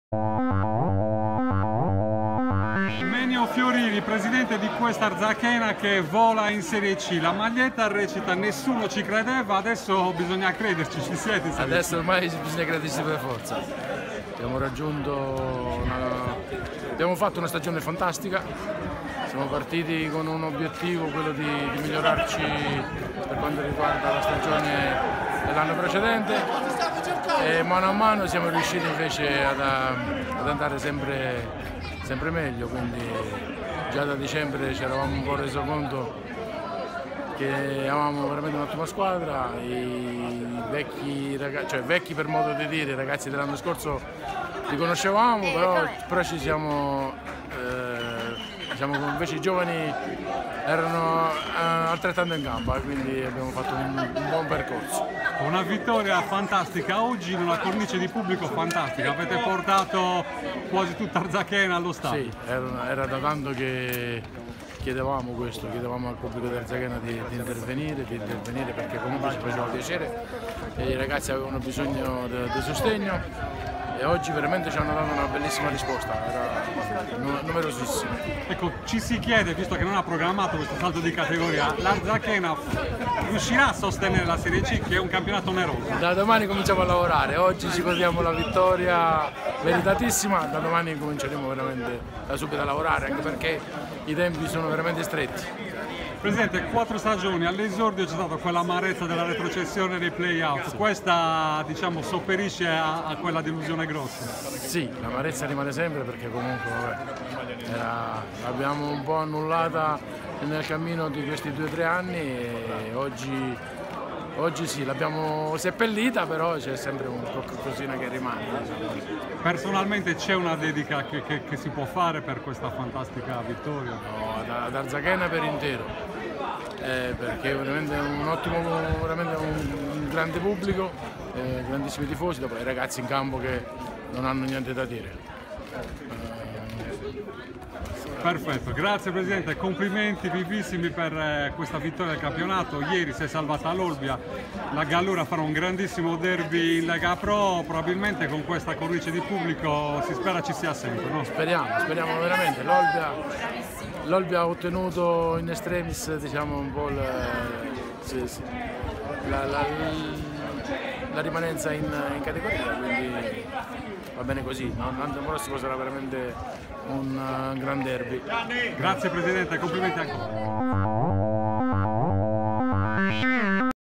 Menio Fiorini Presidente Arzachena 07-05-2017 - Diario Sportivo
Intervista a Menio Fiorini, Presidente Arzachena
Arzachena - Albalonga 4-0
34a giornata Serie D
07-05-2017